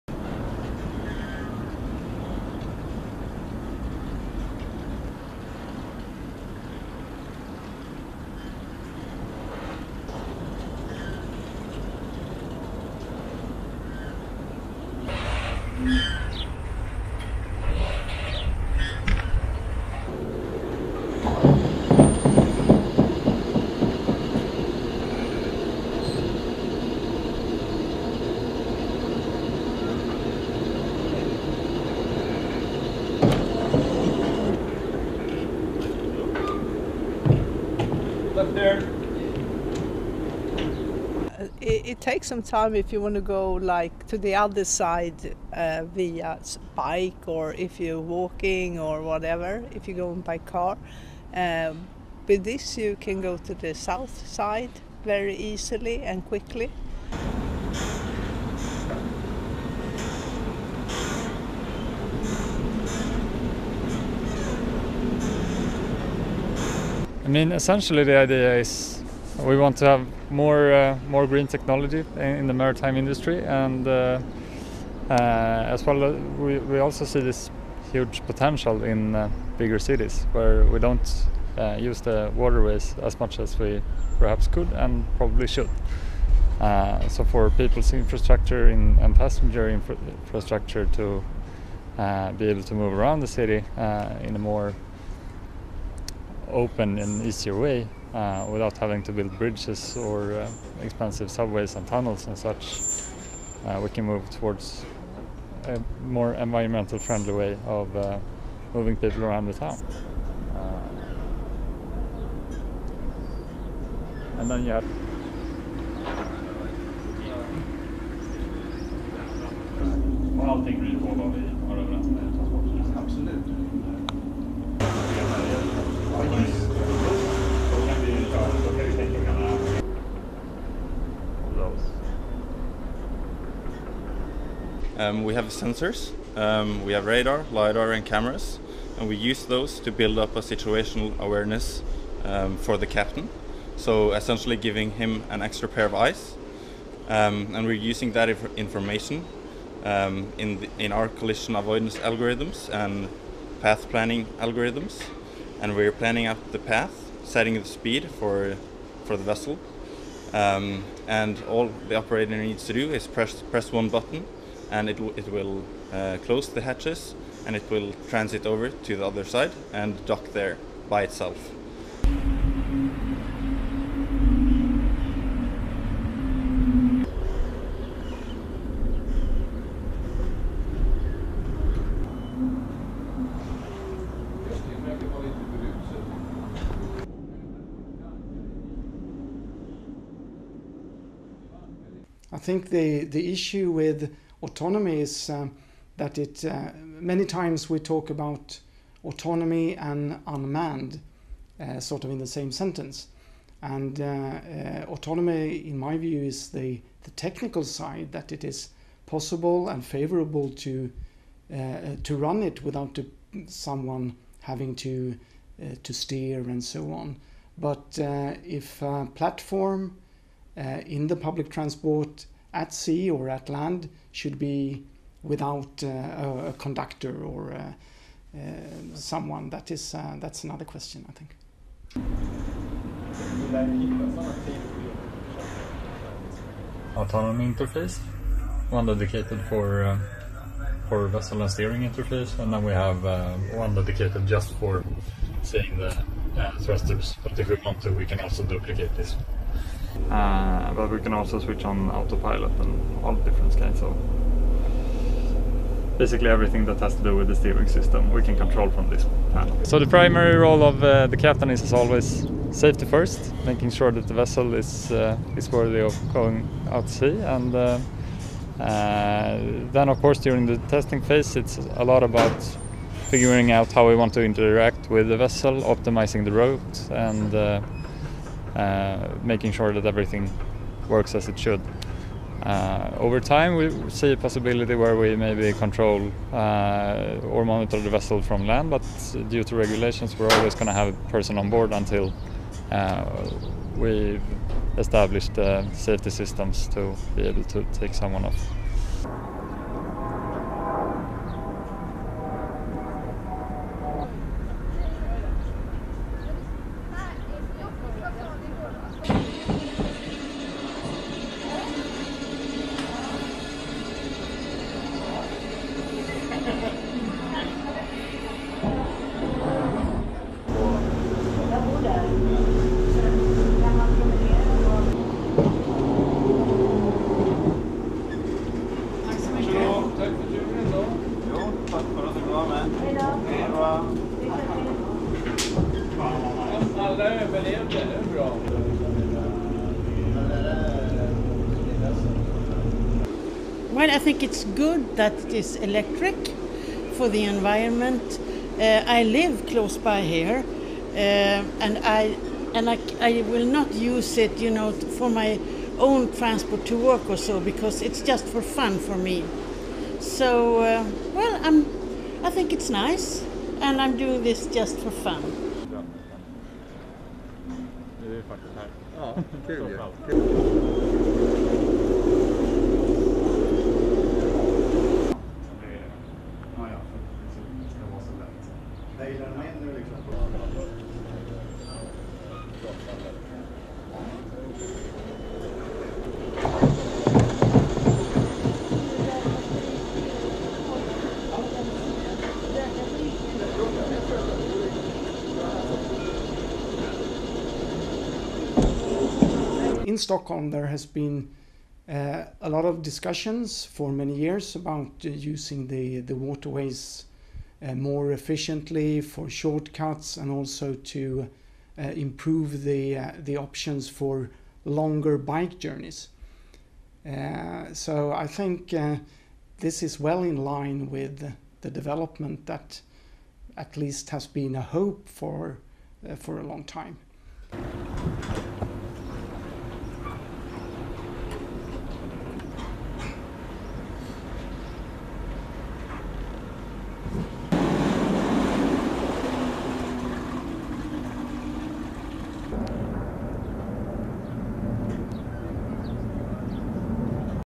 Driverless ferry takes to the water in Stockholm
SWEDEN SELF DRIVING FERRY

RESTRICTION SUMMARY:

LENGTH: 7:27

ASSOCIATED PRESS
Stockholm, Sweden - 12 June 2023

1. Wide of self-driving ferry calculating space to dock
2. Mid of self-driving ferry calculating space to dock
3. Mid of self-driving ferry docking
4. Wide of self-driving ferry docking
5. Mid of ferry opening the hatch
6. SOUNDBITE (English) Lena Bercoff, Stockholm resident:
“It takes some time if you want to go to the other side via bike or if you’re walking or whatever, if you go by car, with this you can go to the south side very easily and quickly.”

7. Wide of poster signalling time left until next ferry
8. Close of poster signalling time left until next ferry
9. SOUNDBITE (English) Erik Nilsson, operational manager, Torghatten
“Essentially, the idea is that we want to have more green technology in the maritime industry, and we also see this huge potential in bigger cities where we don’t use the waterways as much as we perhaps could and probably should. So for people’s infrastructure and passenger infrastructure to move around the city in a more open and easier way, without having to build bridges or expensive subways, and tunnels and such, we can work towards a more environmentally friendly way of moving people around the town.”

10. Mid of radar on top of ferry
11. Close of laser scanner
12. Close of camera
13. Close of electro-optical cameras
14. Mid of ultrasonic sensors to measure distance to the dock
15. SOUNDBITE (English) Eivind Løvoll, engineer, Zeabuz:
“We have sensors, we have radar, LiDAR and cameras, and we use those to build up a situational awareness for the captain, so essentially giving him an extra pair of eyes, and we’re using that information in our collision avoidance algorithms and path planning algorithms, and we’re planning out the path, setting its speed for the vessel, and all the operator needs to do is press one button, and it will close the hatches, and it will transit over to the other side, and dock there, by itself.”

16. Close of reflection on glass of ferry leaving the dock
17. Mid of ferry leaving
18. Wide of view from inside the ferry
19. Mid of view from inside the ferry

ASSOCIATED PRESS
Stockholm, Sweden - 13 June 2023

20. SOUNDBITE (English) Karl Garme, assistant professor in Naval Architecture, KTH Royal Institute of Technology:
“I think the issue with autonomy is that many times, we talk about autonomy and unmanned in the same sentence.  And autonomy, in my view, is the technical side that it is possible and favourable to run it without someone having to steer and so on, but if a platform in the public transport at sea or at land should be without a conductor or someone, that’s another question.”

21. Mid of captain inside captain cabin
22. Tracking shot of captain cabin’s interface
23. Close of screen inside cabin UPSOUND (English) Erik Nilsson, operational manager, Torghatten : “We can also switch on the autopilot, basically everything  that has to do with the steering system can be controlled from this panel.”
24. SOUNDBITE (English) Erik Nilsson, operational manager, Torghatten:

25. Wide of ferry crossing
26. Mid of ferry crossing
27. Tilt down of ferry opening the hatch
28. Mid of view from boat
29. Wide of boat arriving, hatch opening and passengers getting out
30. Mid of passengers enjoying the view
31. SOUNDBITE (English) Margaretta Simm, Stockholm resident:

32. Wide of ferry preparing to dock
33. Close of cameras onboard

ASSOCIATED PRESS